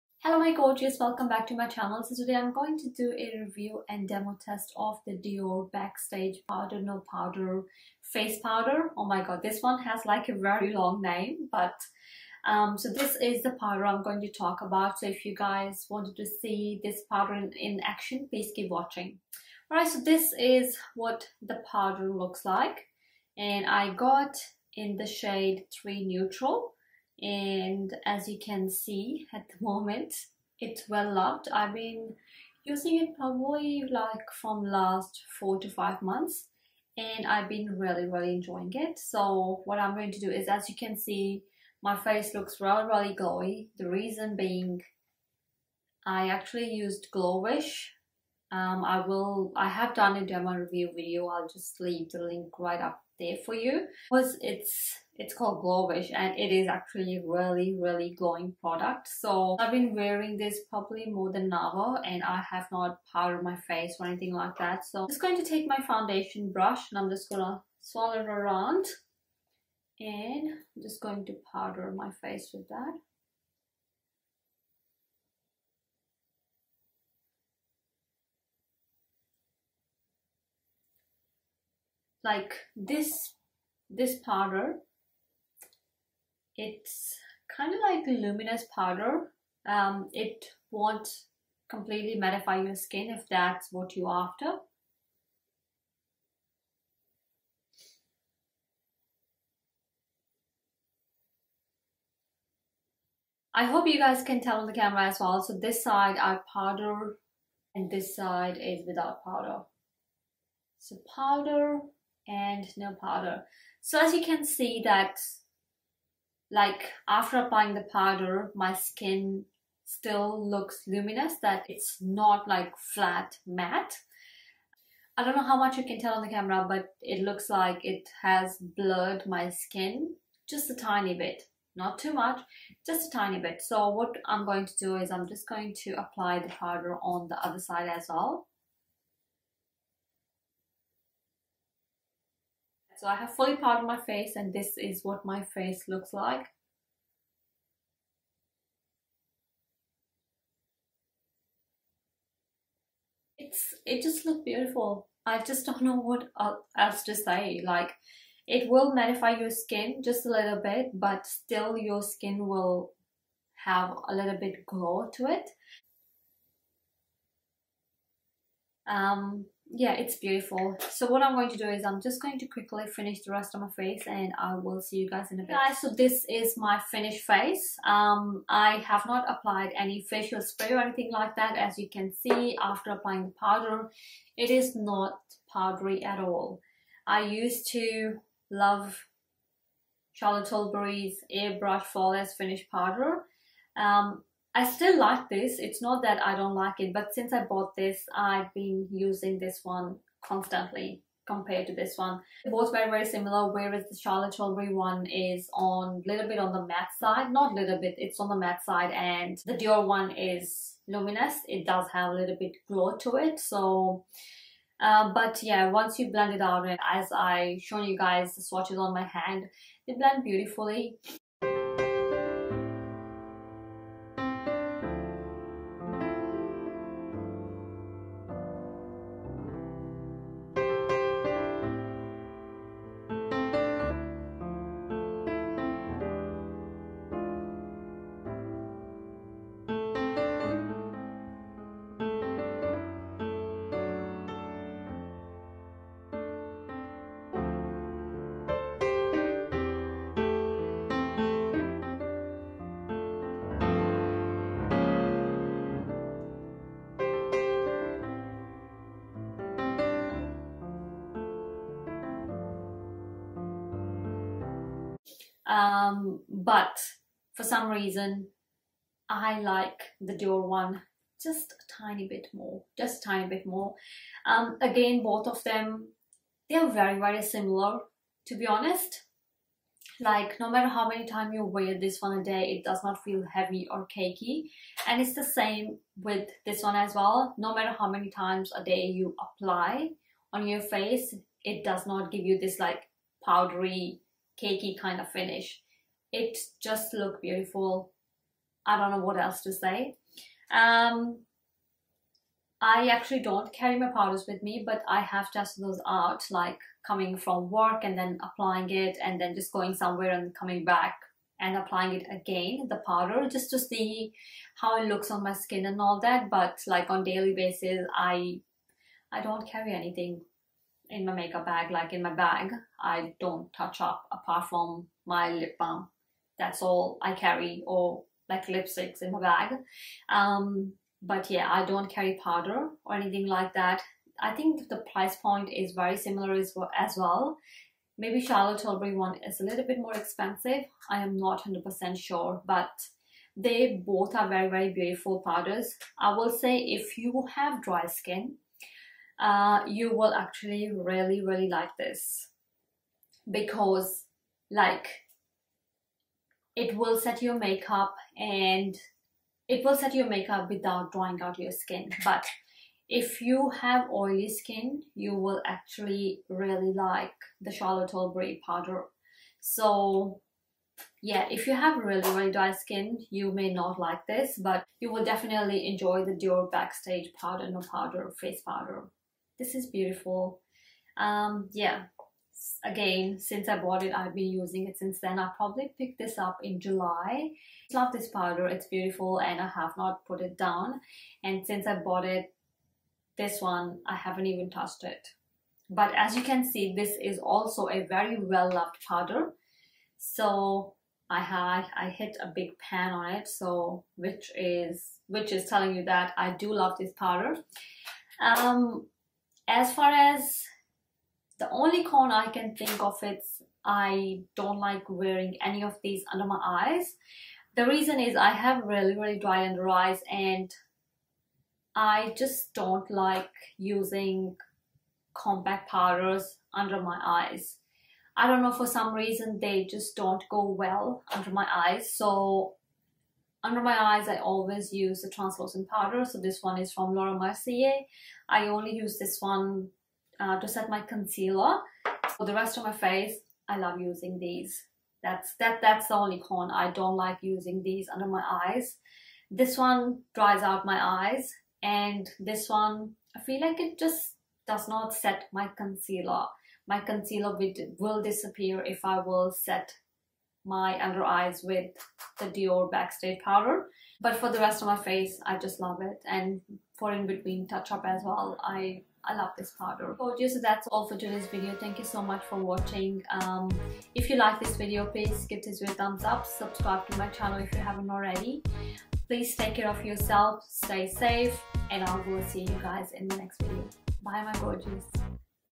Dior Backstage Powder No Powder Review (best powder for Dry, Combo Skin)
Hello my gorgeous today m filming review and demo for Dior backstage powder no powder. Hopefully you guys will like it.My Foundation shades: Mac NC40. Charlotte Tilbury Airbrush Flawless Foundation shade 7, Dior Backstage Foundation 3W. xoxo.

Products Mentioned:-
1. Dior Backstage Powder no Powder: